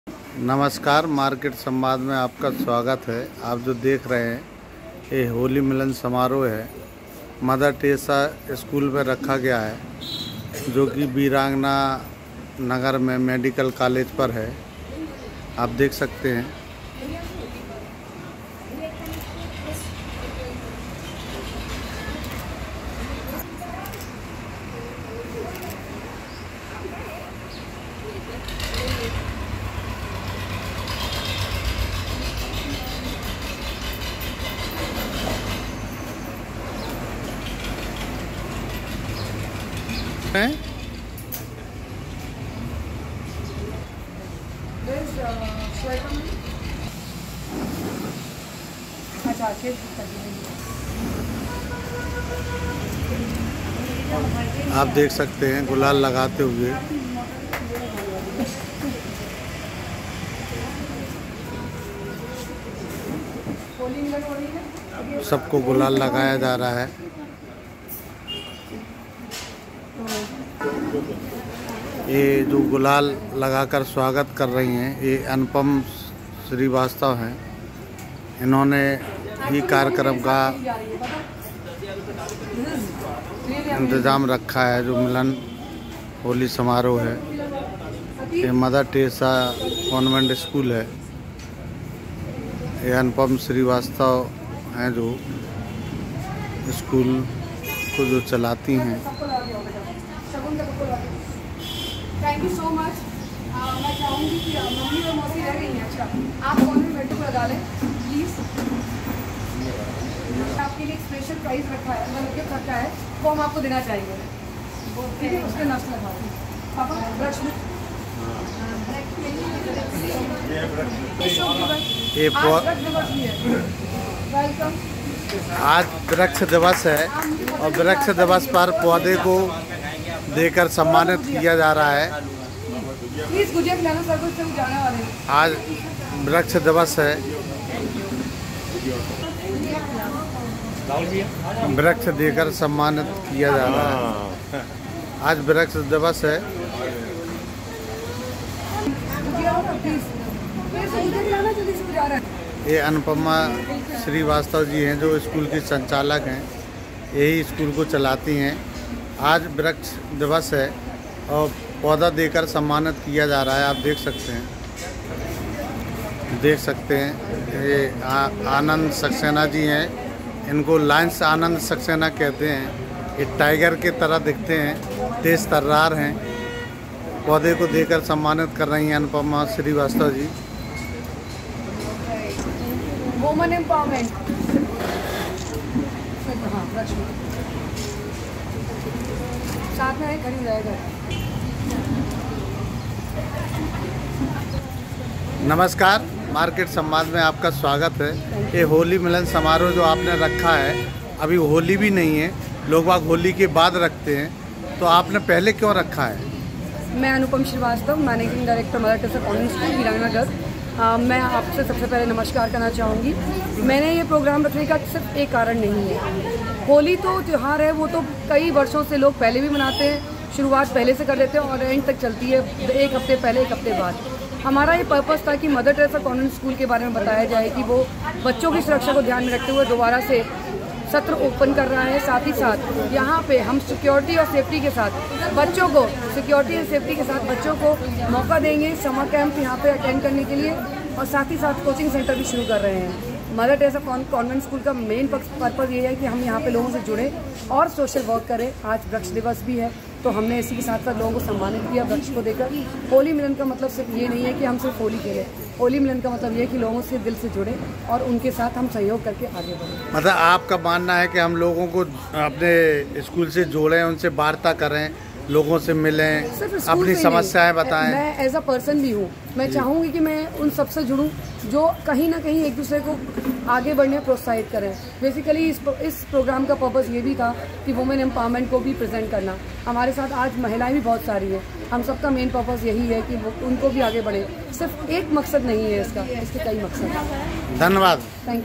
झाँसी-होली की ऐसे हुई शुरुआत, रिपोर्ट-अनिल मौर्य
झांसी में आज शहर में स्थित स्कूल में होली मिलन समारोह का आयोजन किया गया इस समारोह में लोगों ने एक दूसरे से जुड़ते हुए तरक्की के पथ पर आगे बढ़ने का आह्वान किया एक दूसरे को गुलाल लगाया और होली की शुभकामनाएं दी